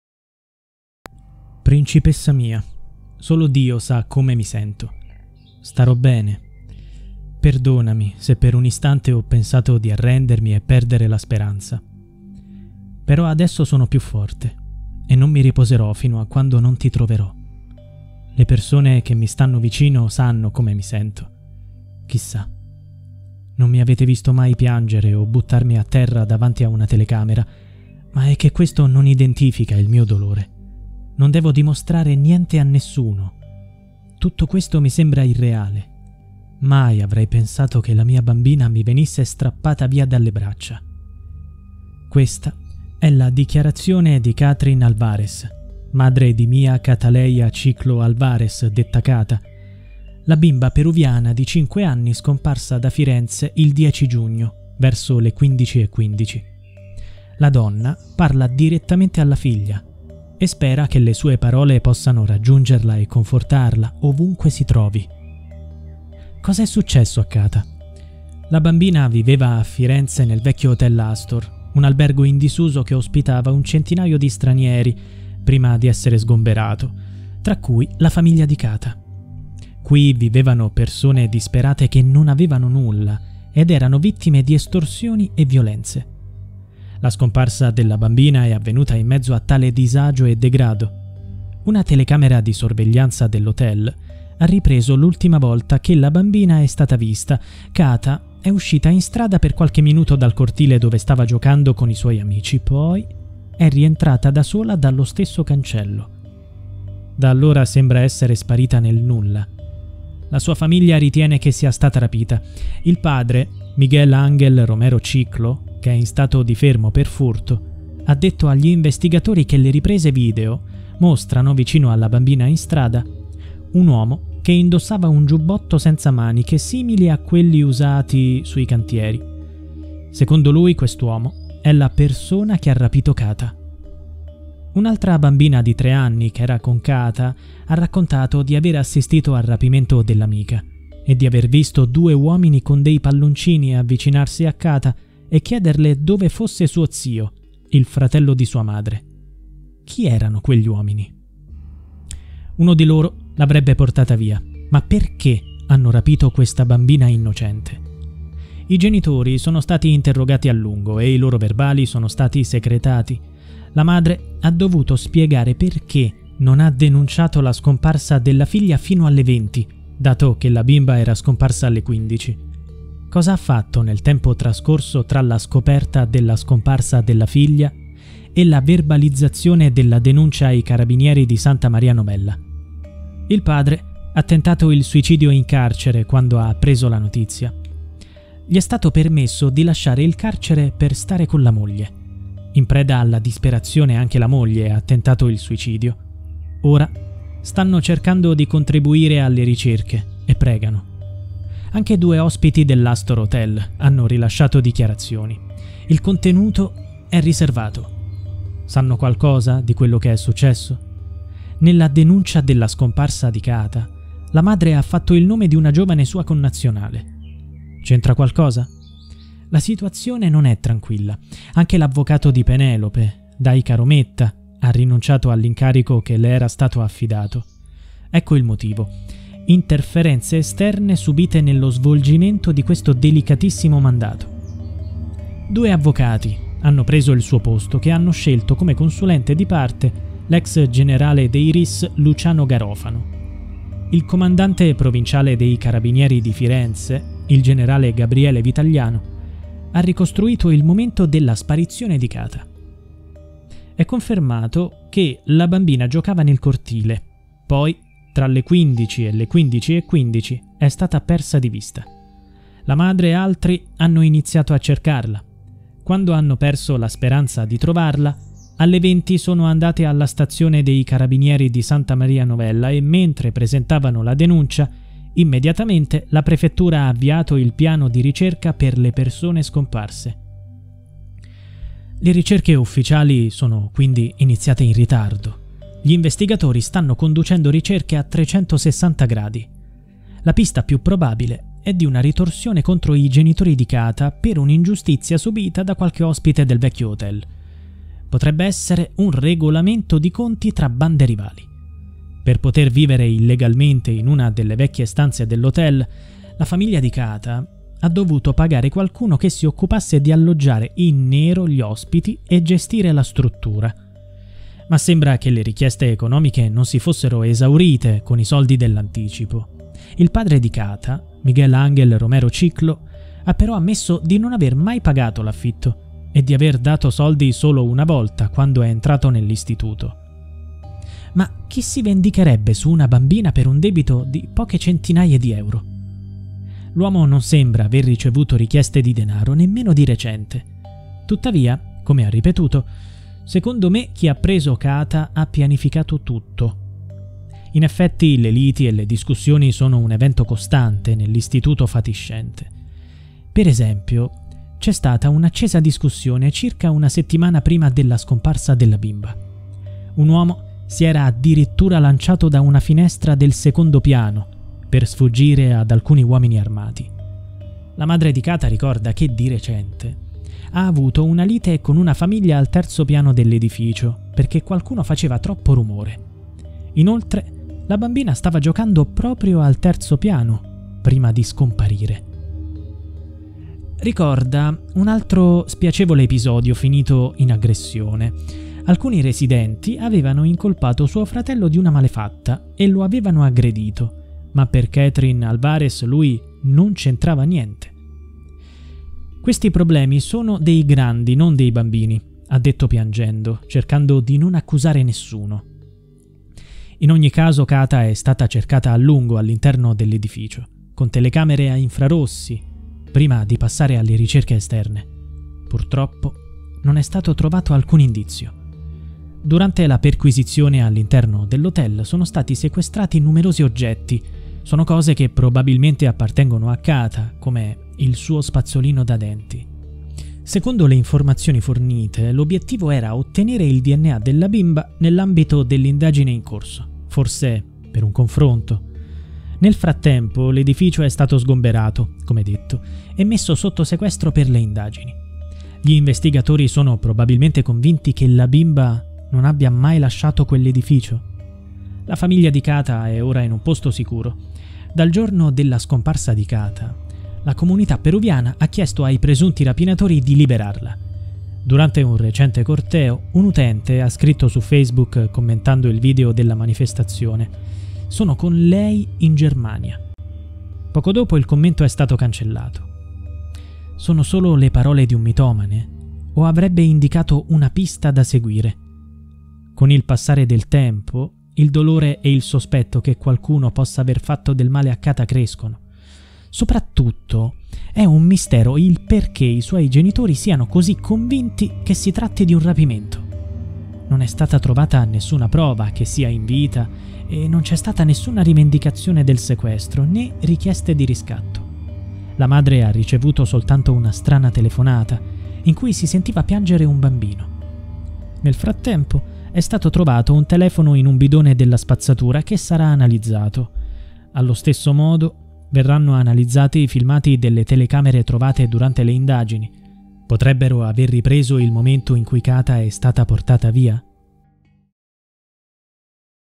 Perché l'hanno rapita? - Kata Alvarez
Il giallo della bambina sparita da Firenze